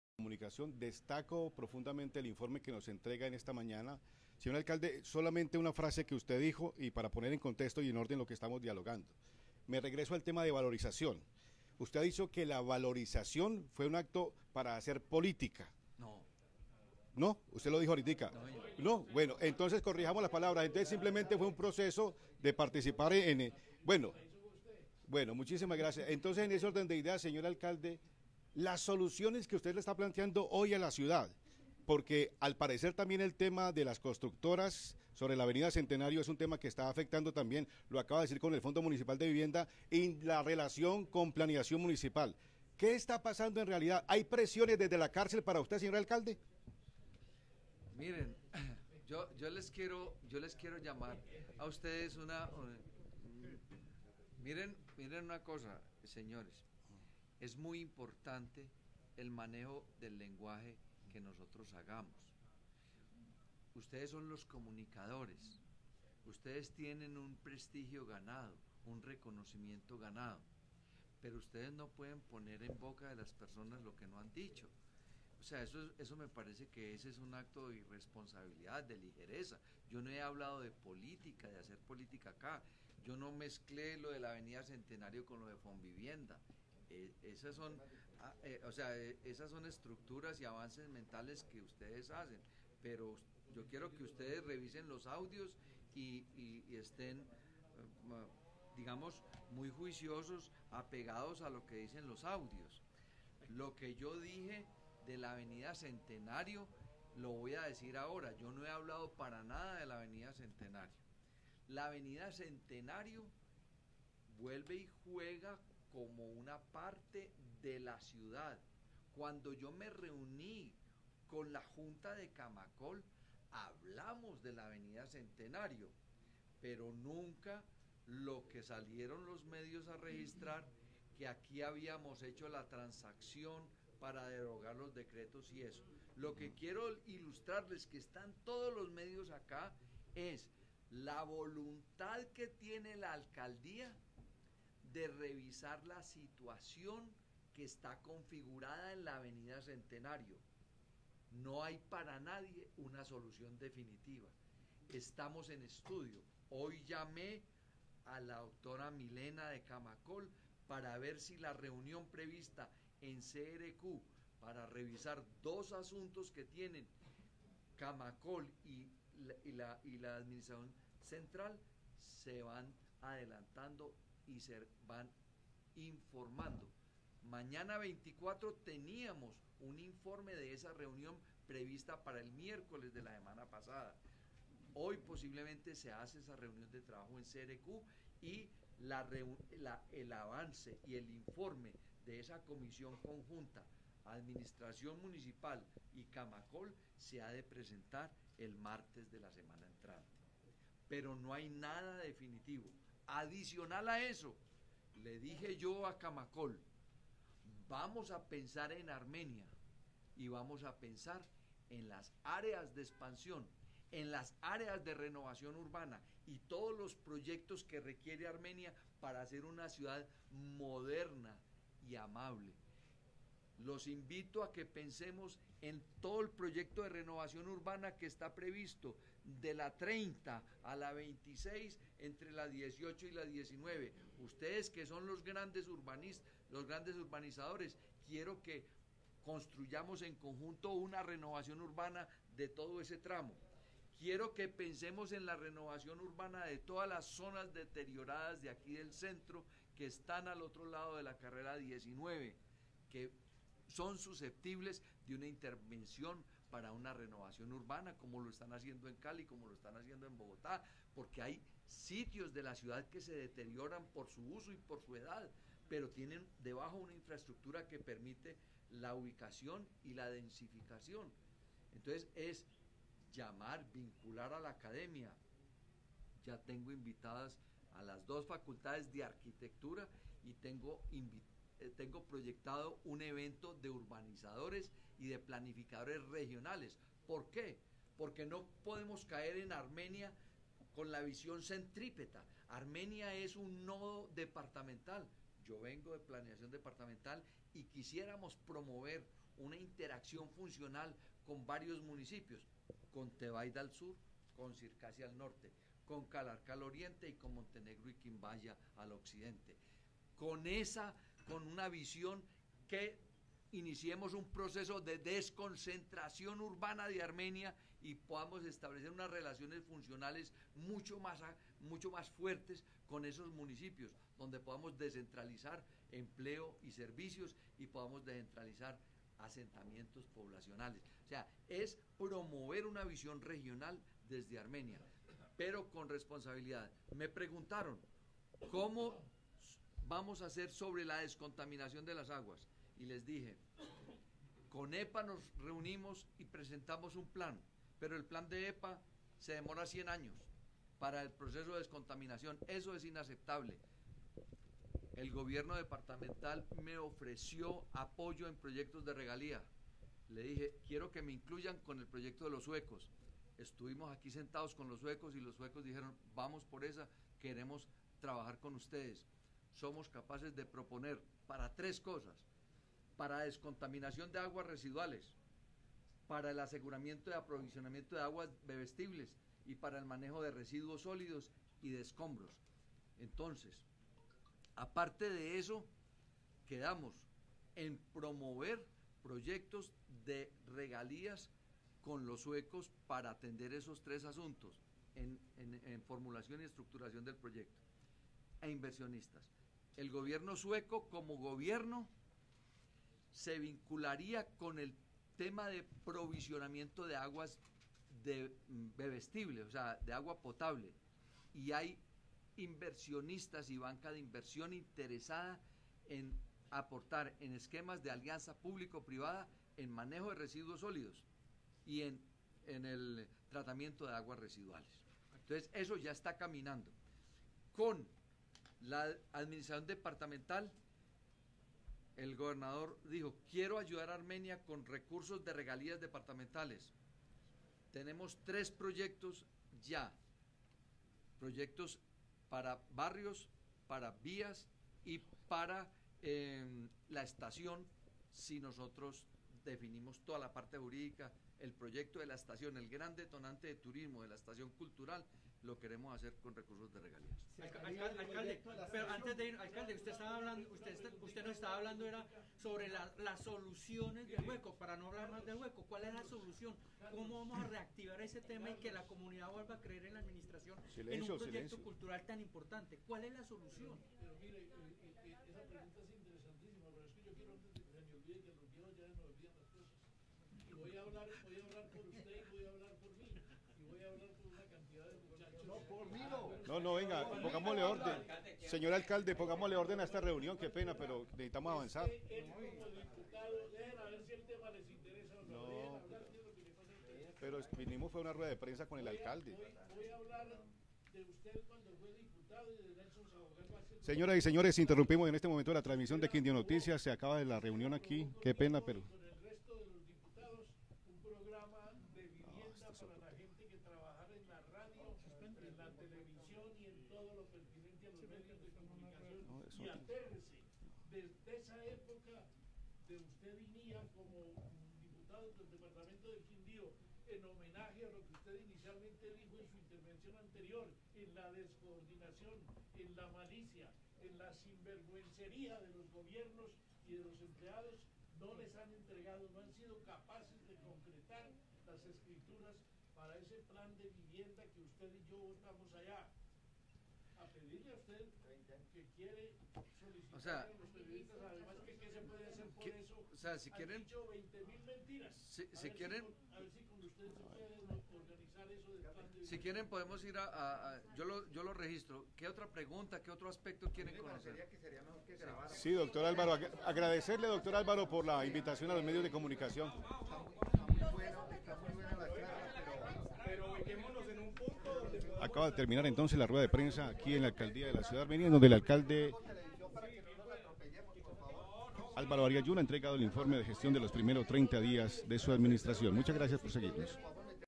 EnVivo Alvaro Arias Young, alcalde de Armenia
EnVivo Alvaro Arias Young, alcalde de Armenia, socializa los resultados de sus primeros 30 días de administración.